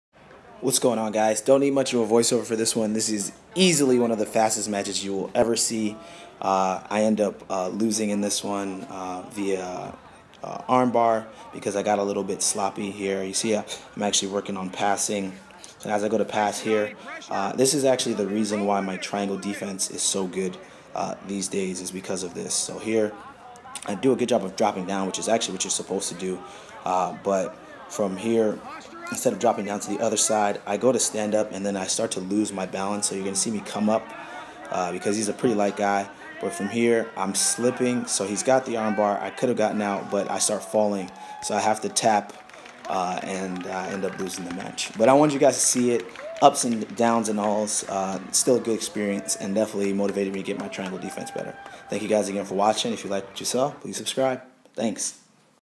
Archives: Shenzhen Tournament Blue Belt| Round 1 | Absolute | November 2016 | BJJ
Taking it back! Here’s a look back at one of my tournaments as a Belt Belt. I give you a look into how my game has developed as a Jiu Jitsu player!